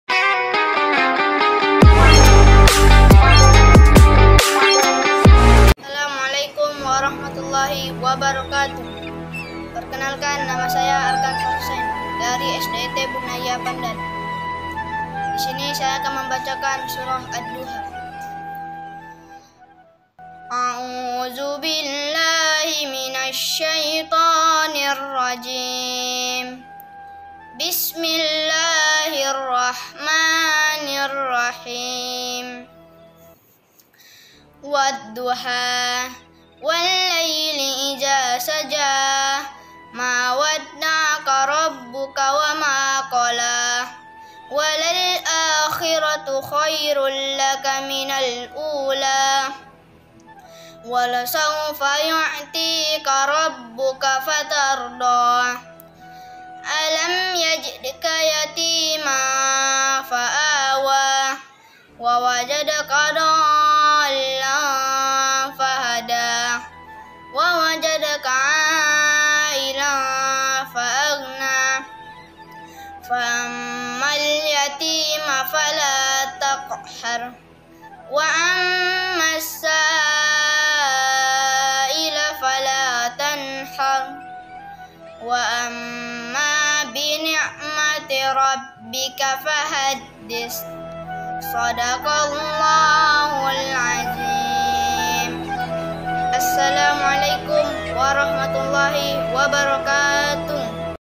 QS. AD-DUHA OLEH ARKAN FAWWAZ HUSAINASAL | SDIT BUNAYYA PANDAN
Ramadhan Berkah🤲
MTS Muhammadiyah 04 Sibolga mengadakan lomba bacaan surah pendek.
NPP 59 ARKAN FAWWAZ HUSAIN